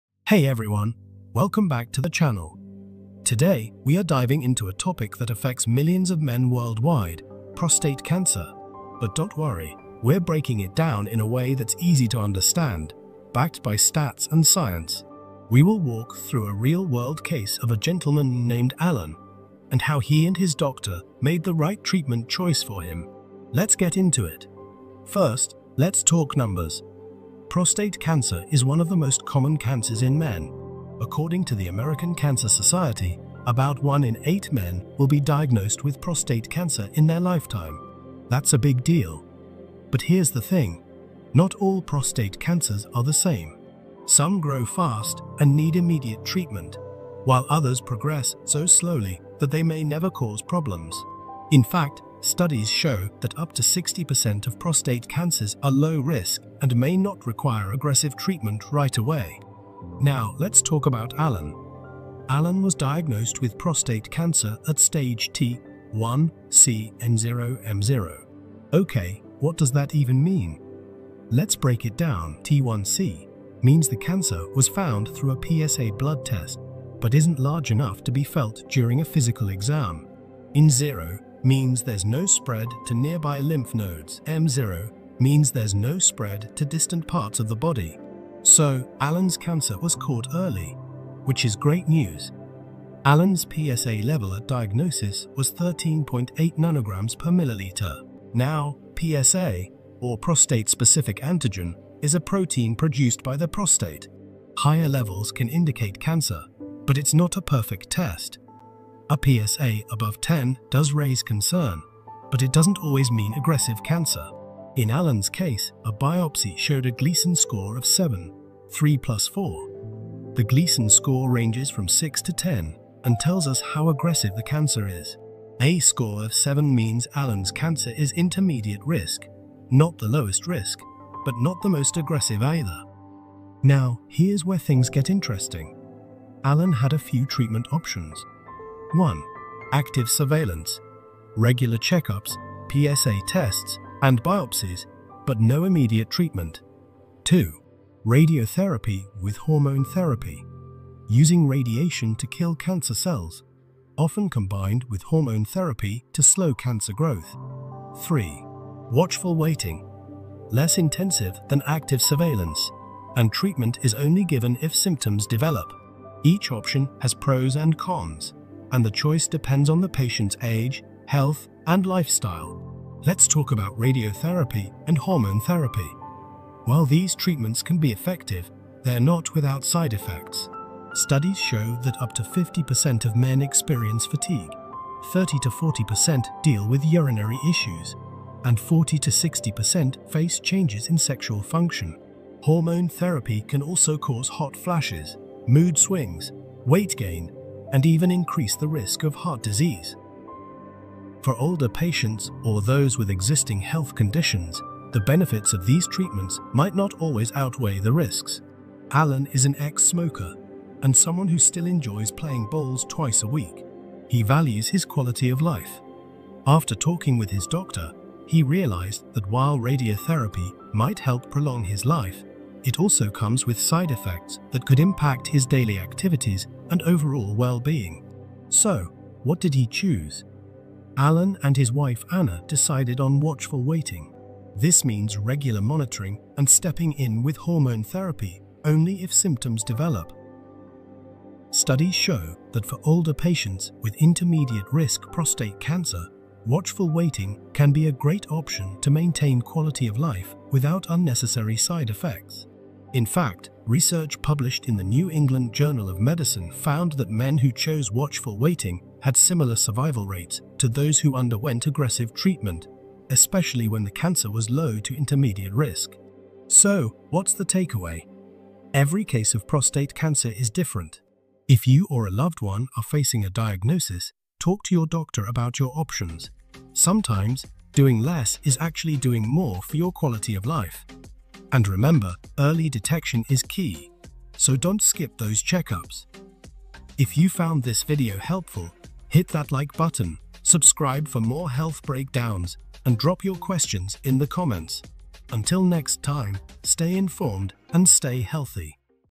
Prostate cancer, To Treat or not to Treat. (A Real-Life Story!).
In this video, we'll discuss the difference between watchful waiting and radiotherapy for prostate cancer treatment. Hear a real-life story and learn about the pros and cons of each option.

0:29 Prostate Cancer Incidence
1:01 Alan's Diagnosis
1:42 What is a PSA ?
1:59 Gleason score explanation
2:20 Treatment Options
2:55 Radiotherapy and hormones pros and cons
3:53 Alan's Decision
4:29 Take away message